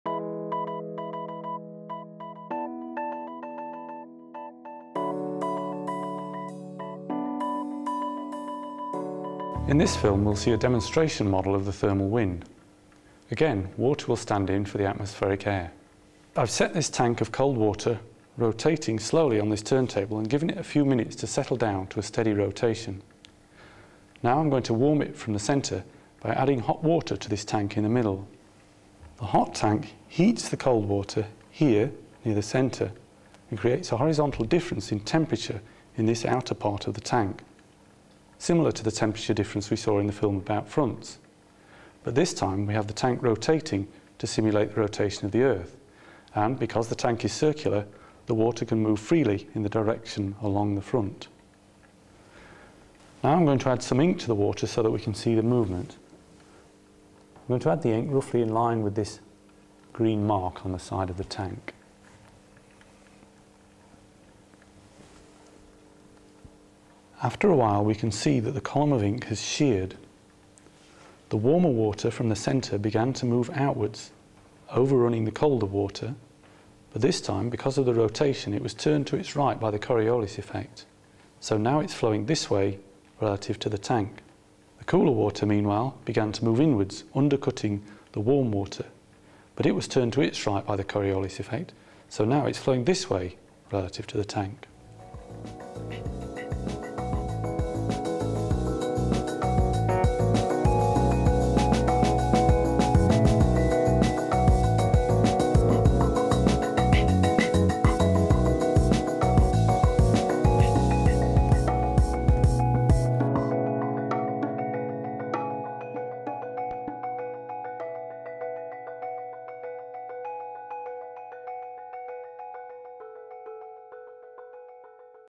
Thermal wind demonstration
In this series of videos, Tom Howard shows how to create different experiments to explain the science behind weather.

In this video, Tom explains an experiment that demonstrates the thermal wind.